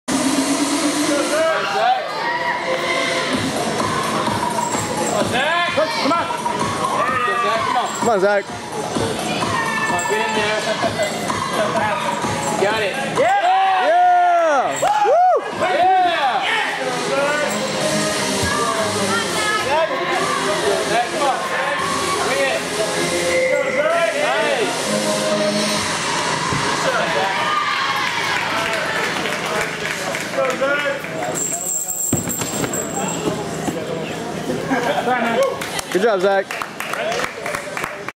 2015 TTU Zach Bennett M HB
Zach Bennett's high bar routine from Texas Tech's gymnastics meet hosted on February 21, 2015.